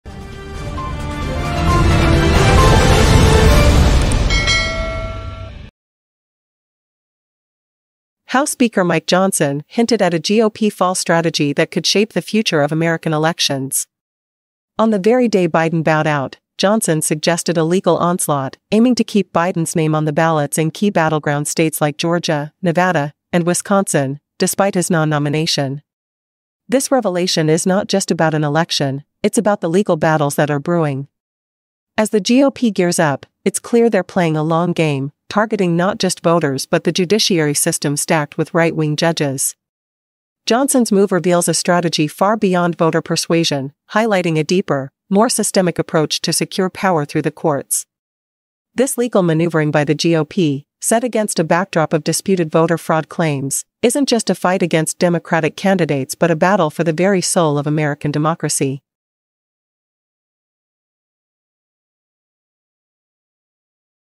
Exposed: GOP's Secret Weapon Against Biden’s Exit!
House Speaker Mike Johnson's remarks suggest a legal showdown aimed at keeping Biden's name on the ballot across pivotal states. This isn't just politics as usual; it's a calculated move to engage the judiciary, armed with judges sympathetic to the right-wing cause. The strategy exposes a plan to challenge the election results well beyond November 5, setting the stage for a legal saga that could ultimately decide who takes the White House.

GOP | election 2024 | legal strategy | Mike Johnson | voter fraud | Supreme Court | Federalist Society | election integrity | battleground states | judiciary

Category:
Politics